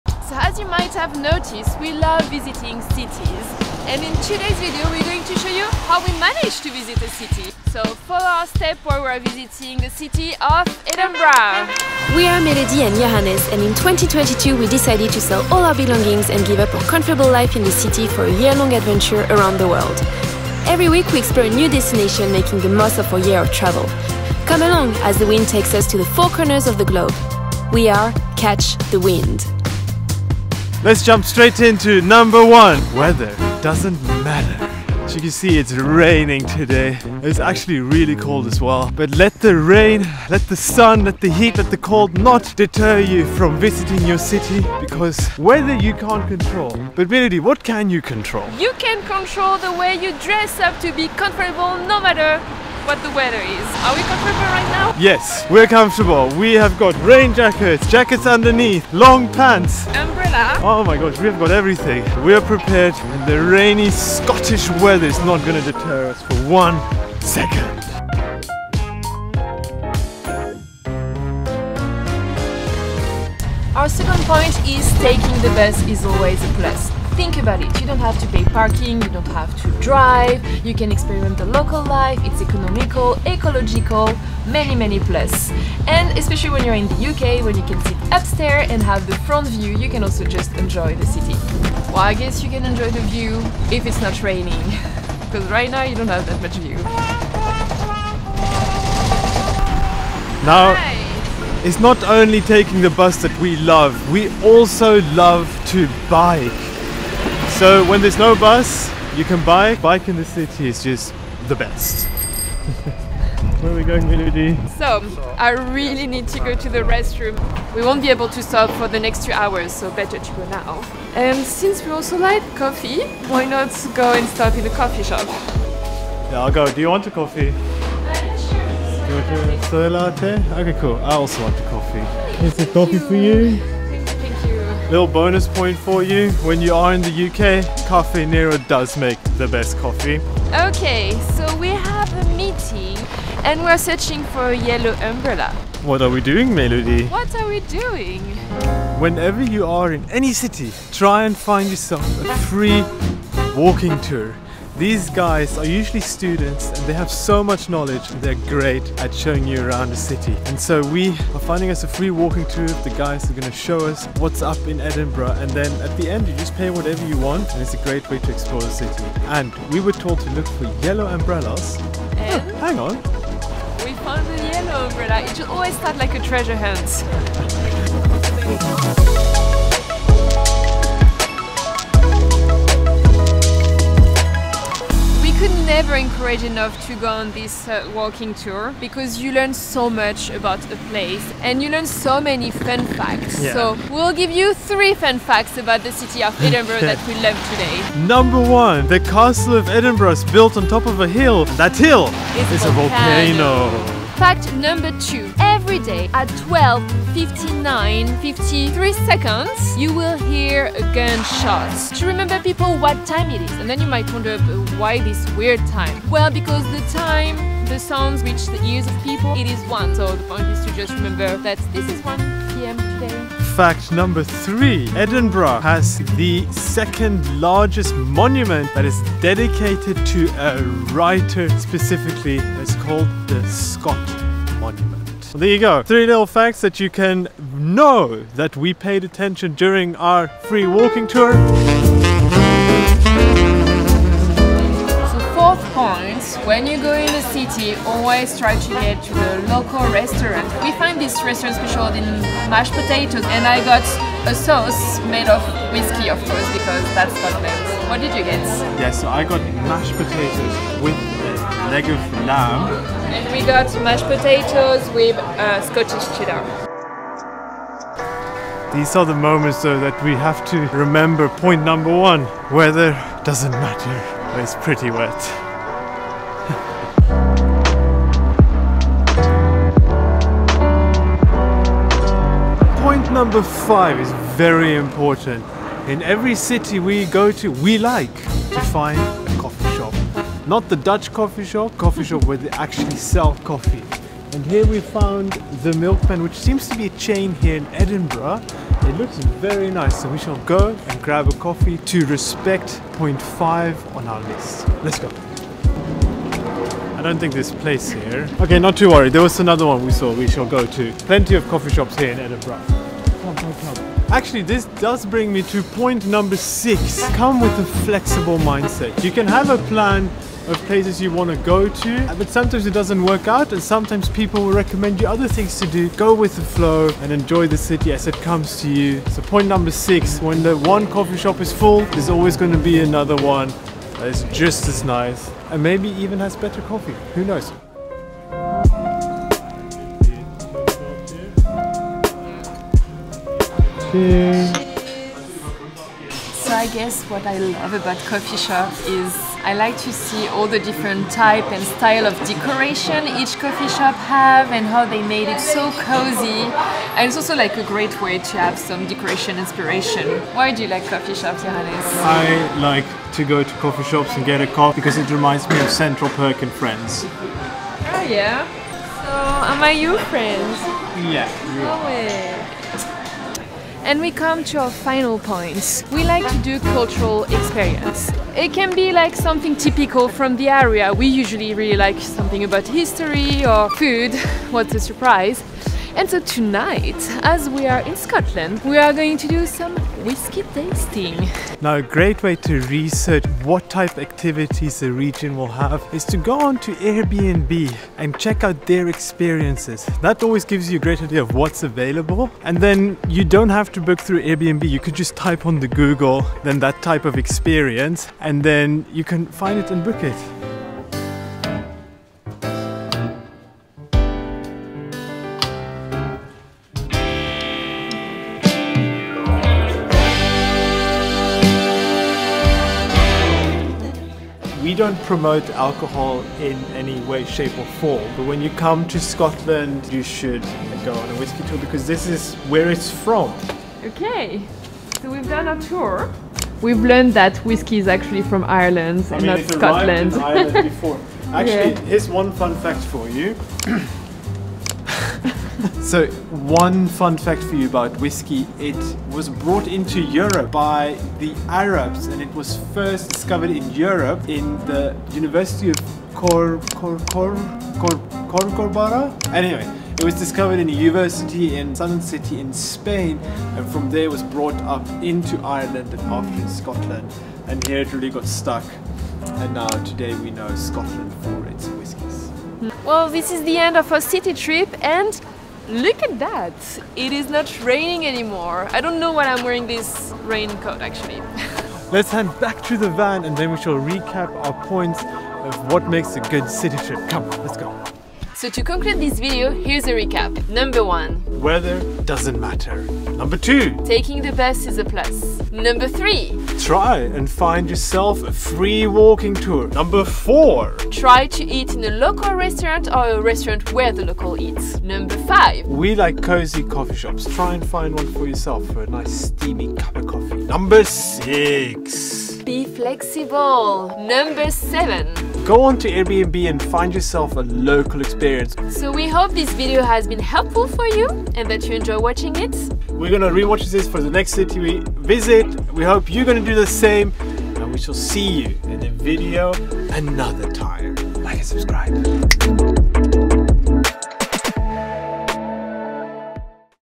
How to visit any city - Edinburgh, Scotland
This video guide gives you our best tips and tricks to make every city trip memorable and unique.
We have a list of 7 items we keep in mind every time we visit a new city.
Follow us on this rainy day in Edinburgh as we put our list to the test.

Episode 31 | May 2022 | Edinburgh, Scotland